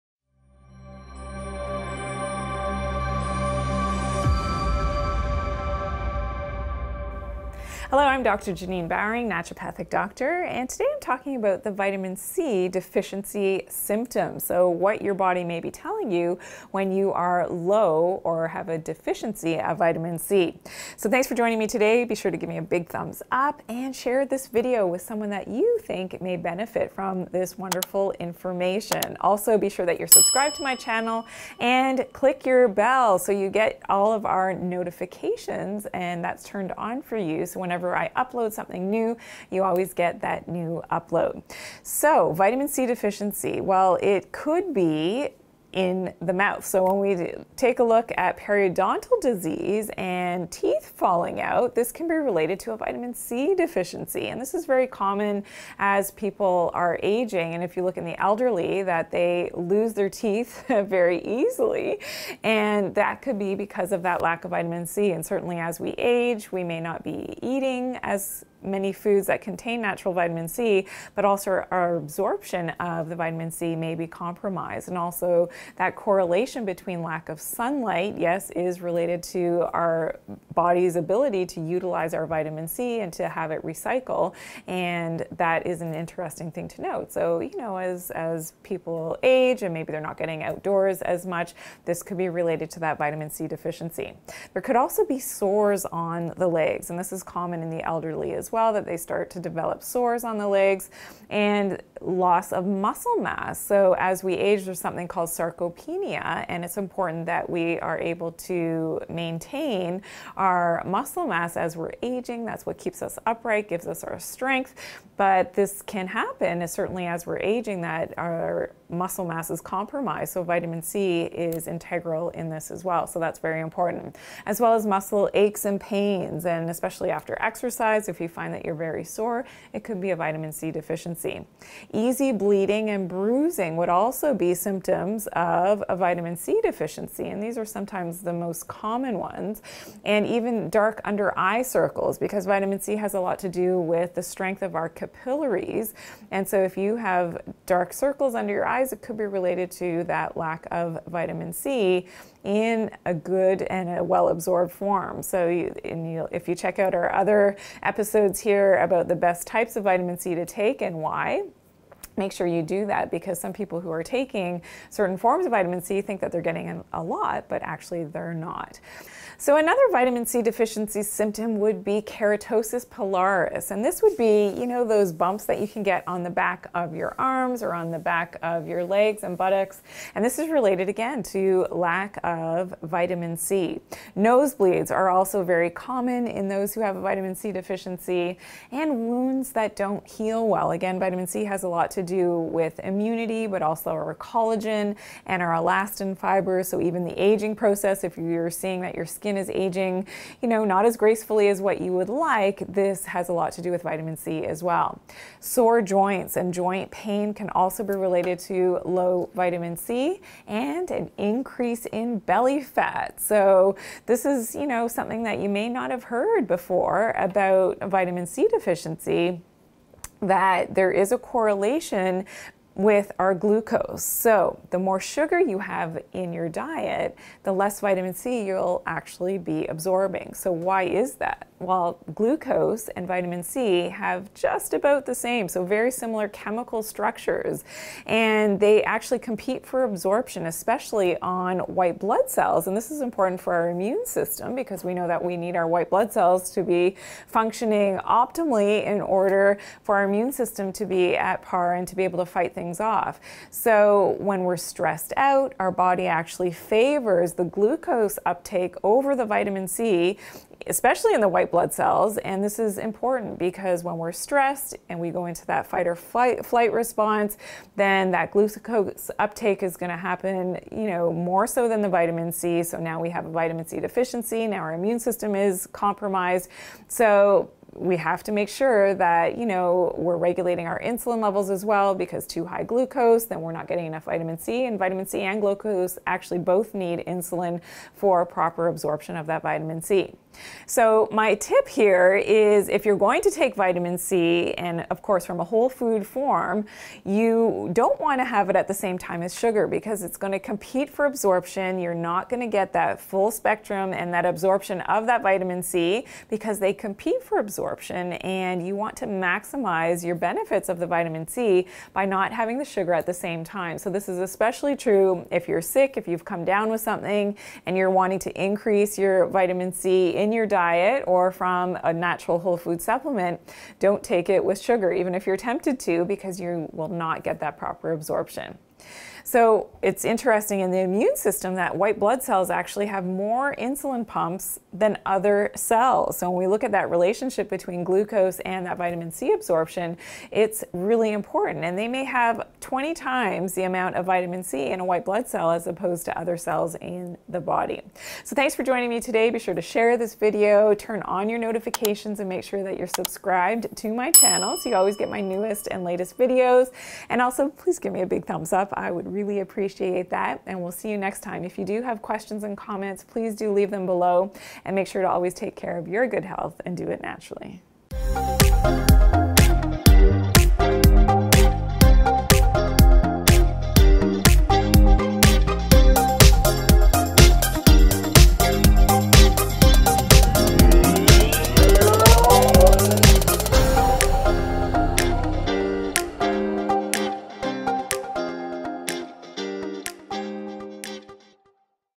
Symptoms Of Vitamin C Deficiency - Dr. J9Live
In this segment of the Dr. Janine Show Dr. Janine will be discussing the Symptoms Of Vitamin C Deficiency.  She will give you a brief history of vitamin c and the importance of vitamin C for the body.  Dr. Janine will explain Vitamin C deficiency symptoms and what you haven’t heard about Vitamin C.  She explain the best and worst vitamin C’s and why synthetic vitamin C is not easily absorbed by the body and the benefits of taking a whole food natural source.  Dr. Janine will explain the side effects of taking too much vitamin C and how to boost your immunity naturally with vitamin C.  She will also tell you 5 impressive ways vitamin C benefits your body and the top 10 vitamin C foods.  As always Dr. Janine will share with you the Dr. J9 Truth about the invention of vitamin C.  She will also be sharing a recipe for how to make a vitamin C serum at home for youthful, glowing and spotless skin.